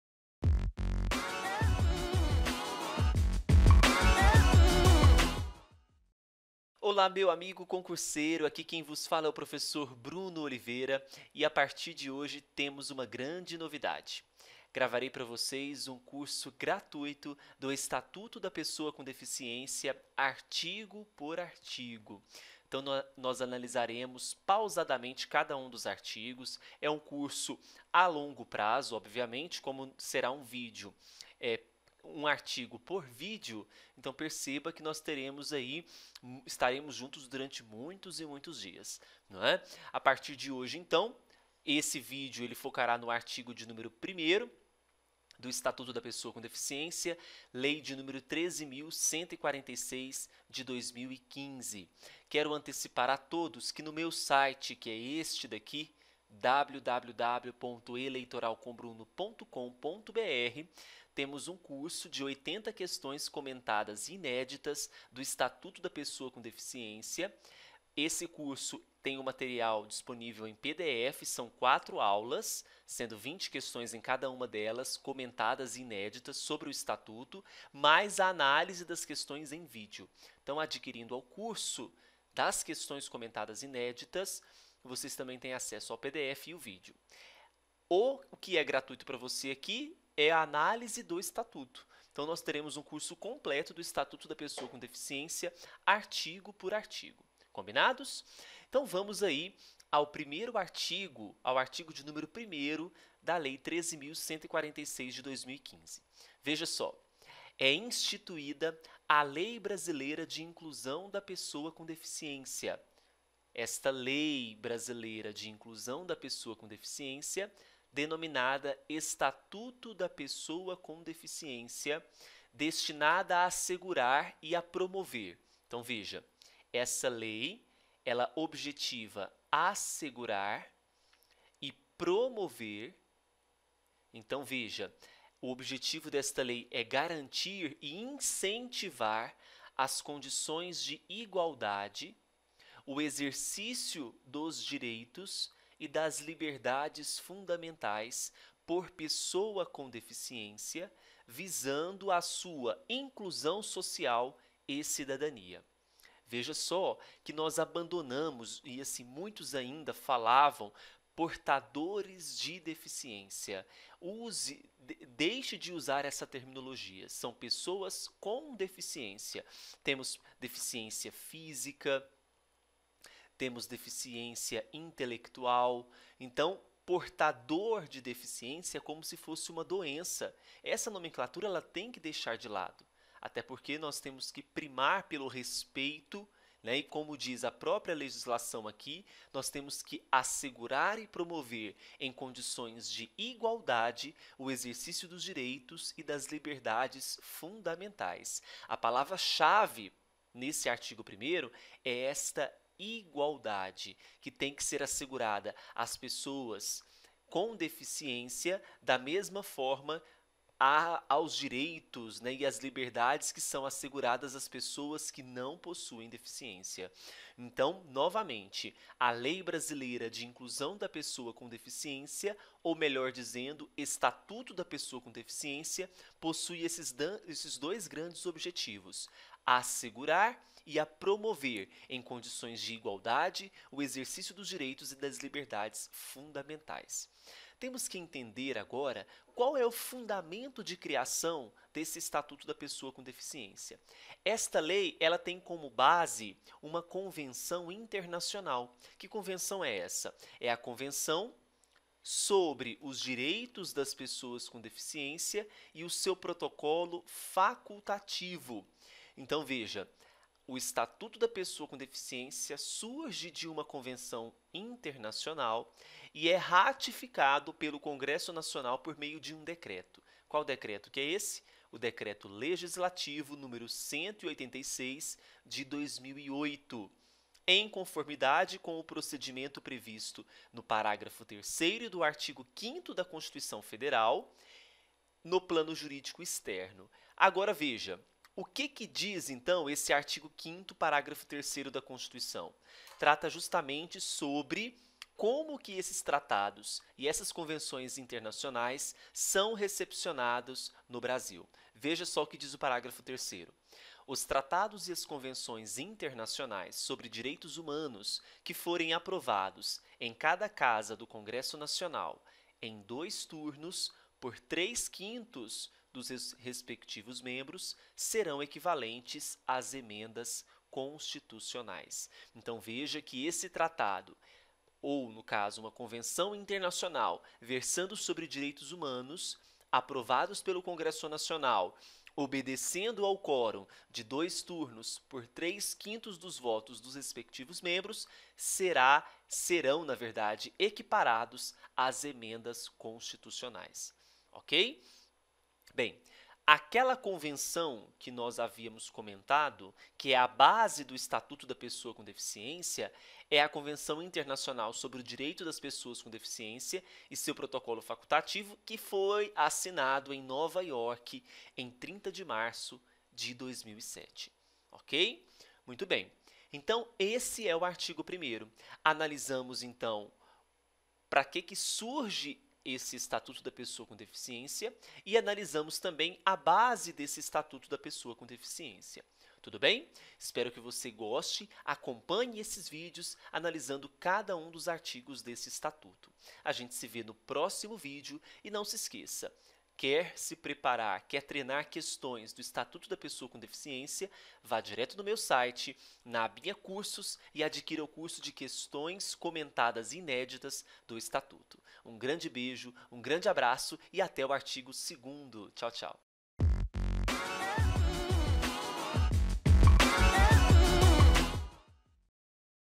Estatuto da Pessoa com Deficiência - Art. 1º - [Curso Gratuito]
ATENÇÃO ! Esse curso foi gravado em 2017, portanto houve algumas alterações na lei. Por isso, sugiro o acompanhamento das aulas em conjunto com a lei seca. Um grande abraço! Professor Bruno!!!